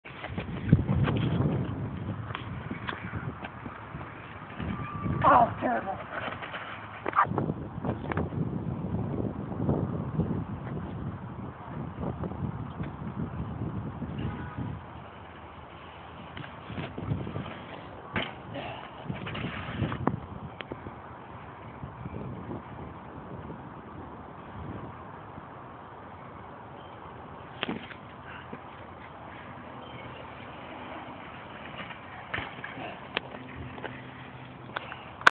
Jeff on a bike
jeff trying to get air on a jump/hill